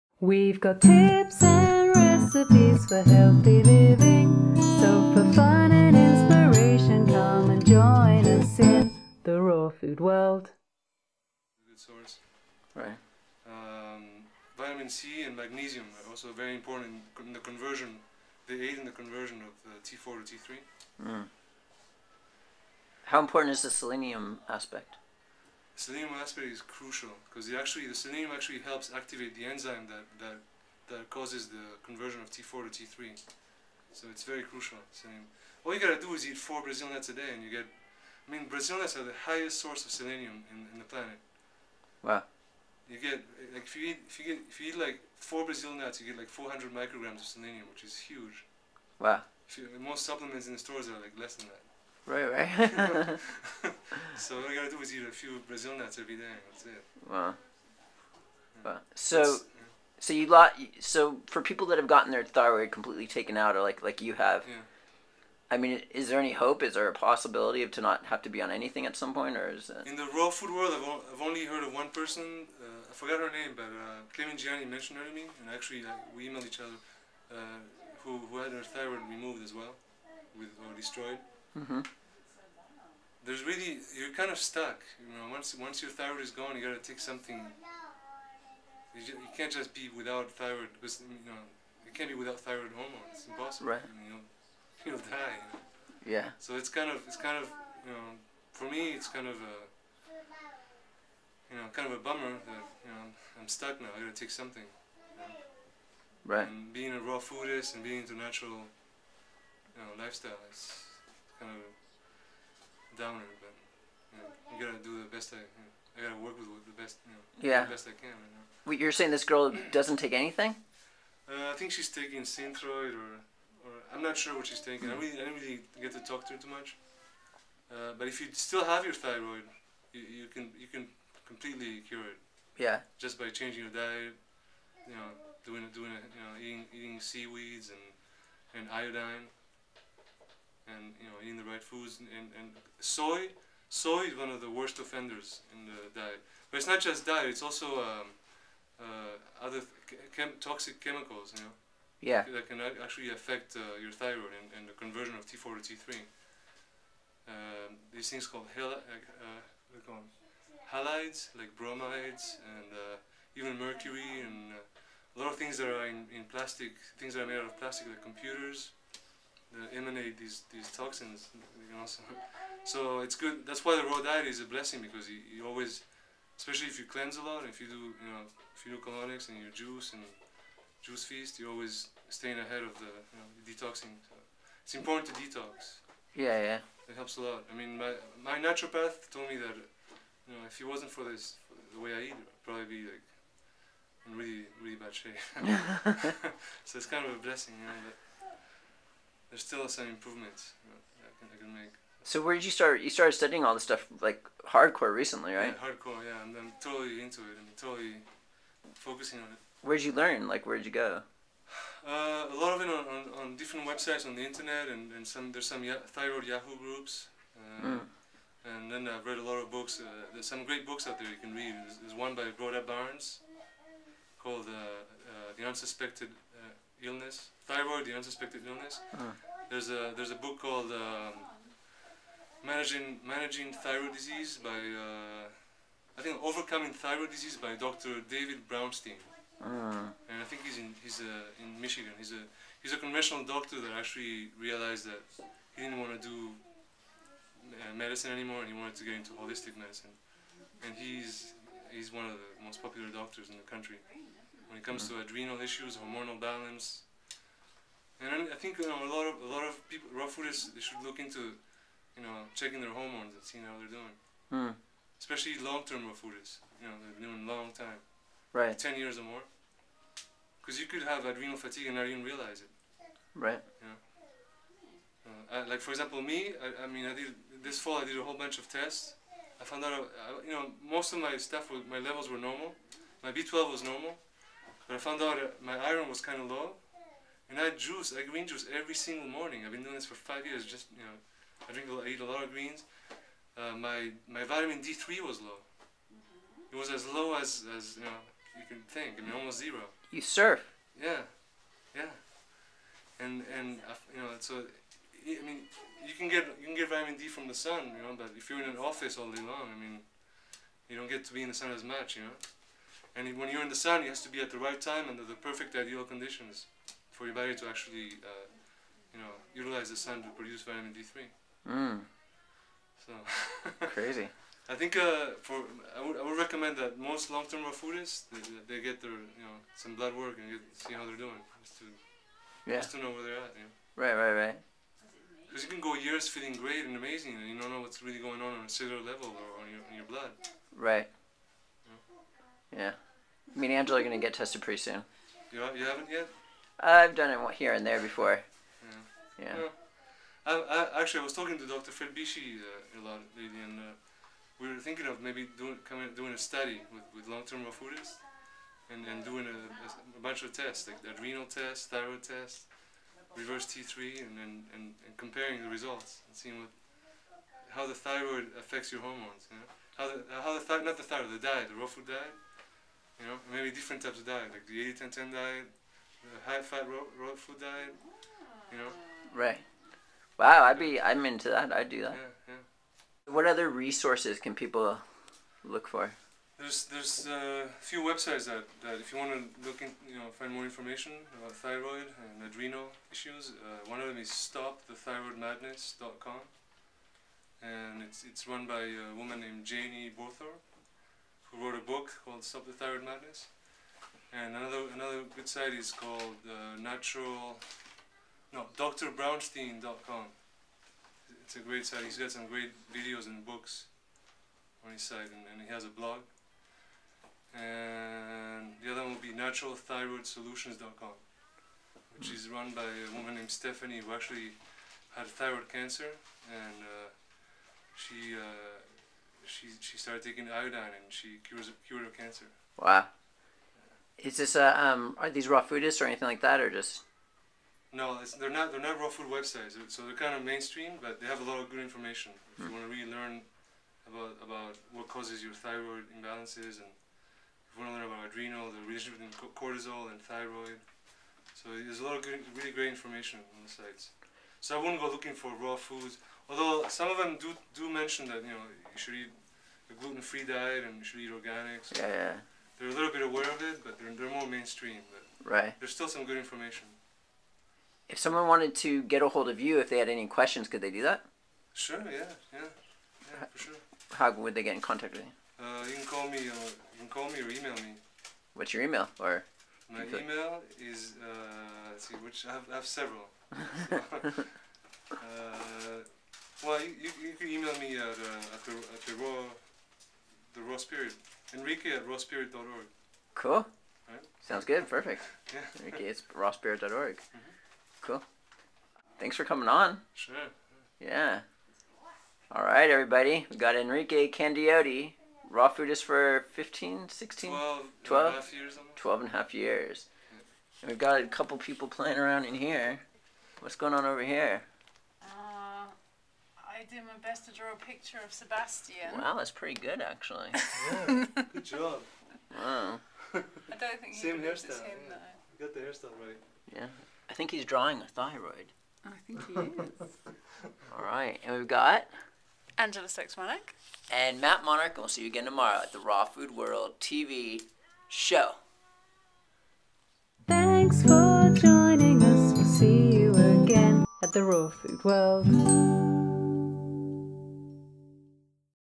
Nutrients that Support the Thyroid (Part III of III), #332
Our friend Enrique discusses the best nutrients that support the thyroid and also discusses references to learn more about your thyroid.